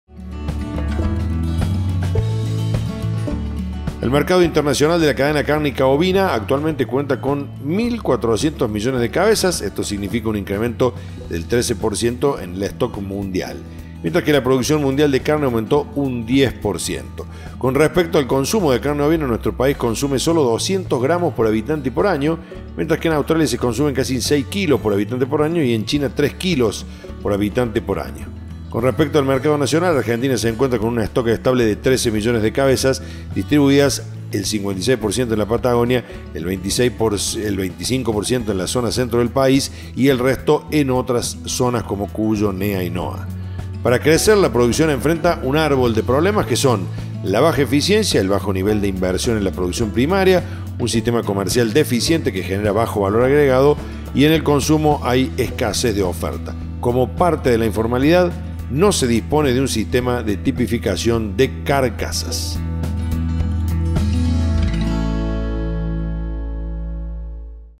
LOS NUMEROS DE LA CARNE OVINA
El mercado internacional de la cadena cárnica ovina actualmente cuenta con 1400 millones de cabezas, esto significa un incremento del 13% del stock mundial. Mientras que la producción mundial de carne aumentó un 10%.  Con respecto al consumo de carne ovina nuestro país consume sólo 200 gs/hab/año, mientras que en Australia se consumen 6kg/hab/año y en China 3kg/hab/añoCon respecto al mercado nacional, Argentina se encuentra con un stock estable de 13 millones de cabezas distribuidas el 56% en la Patagonia y el 25% en la zona Centro del país. Para crecer la producción enfrenta un árbol de problemas" que son: baja eficiencia y bajo nivel de inversión en la producción primaria; un sistema comercial deficiente que genera bajo valor agregado y, en el consumo hay una escasez de oferta. "Como parte de la informalidad, no disponemos de un sistema de tipificación de carcasas